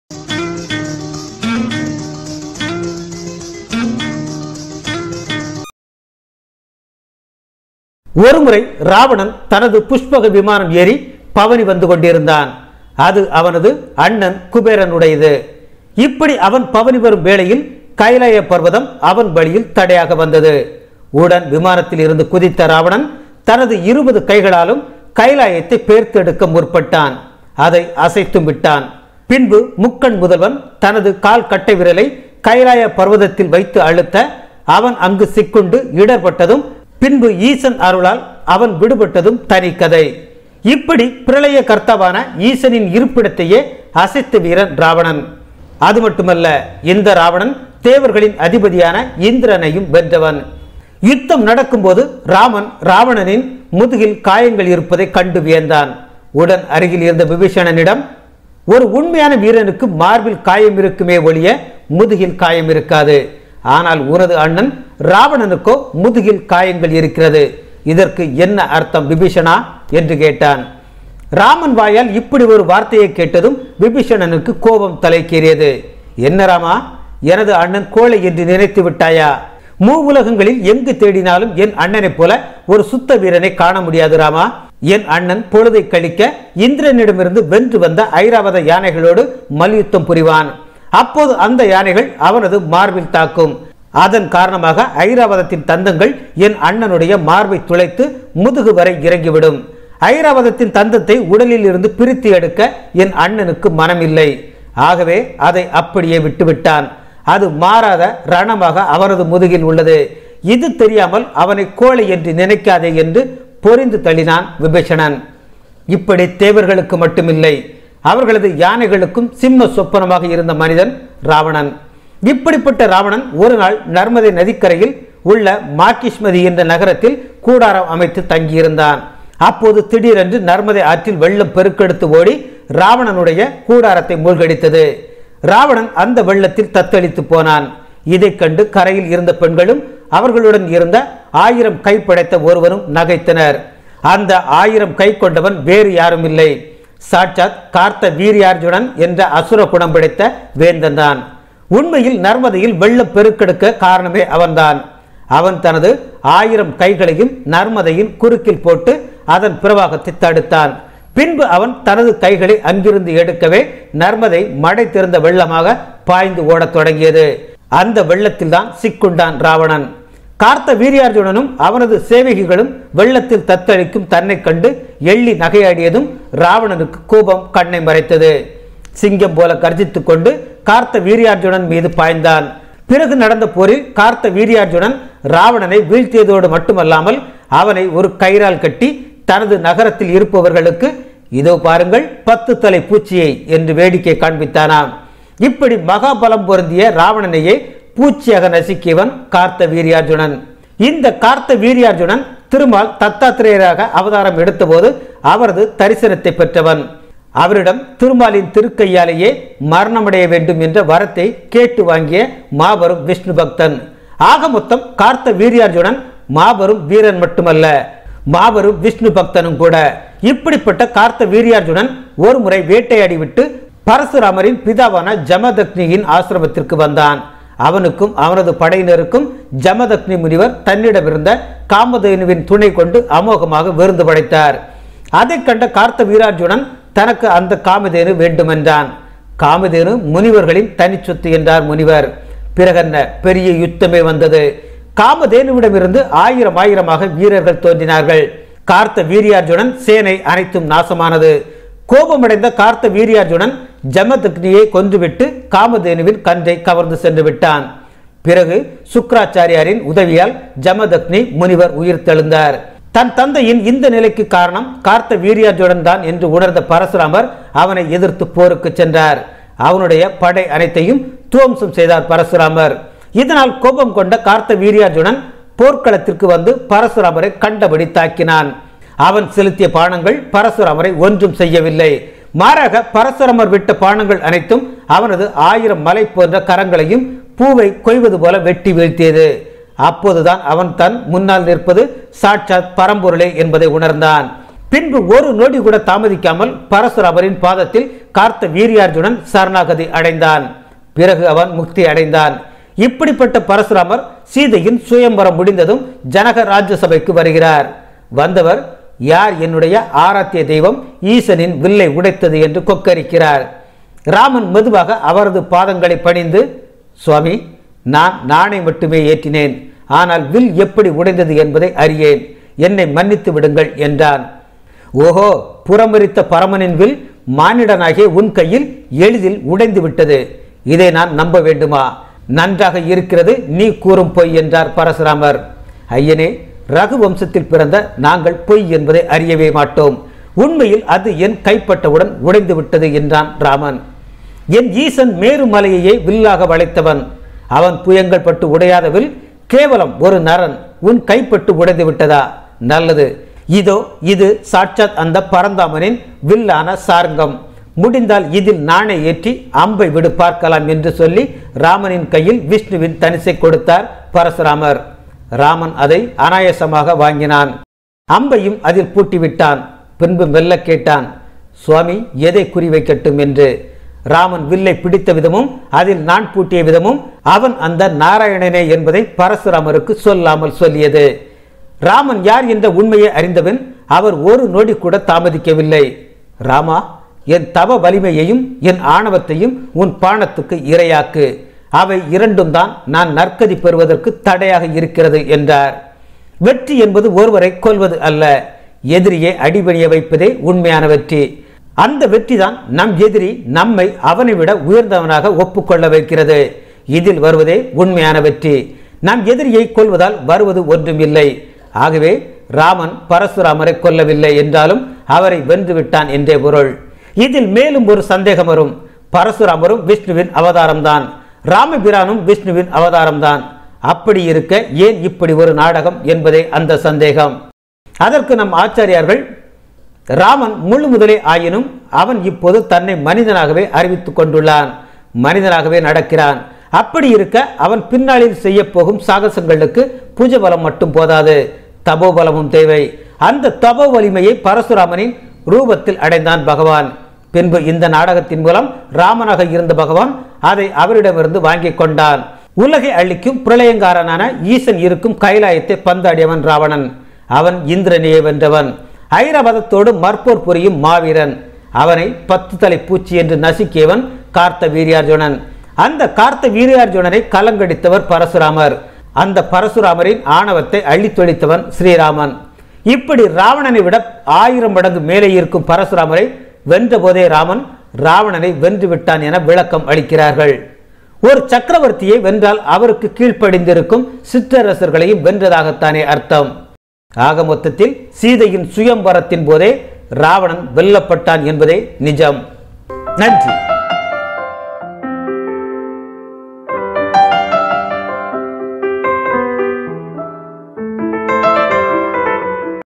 சீதையின் சுயம்வரத்தின் போதே இராவணனை வீழ்த்திய இராமன் | ஆச்சரியமாக இருக்கிறதா?
Seethaiyin Swayamvarathin Pothe Ravananai Veezhthiya Raman Acharyamaga irukkiratha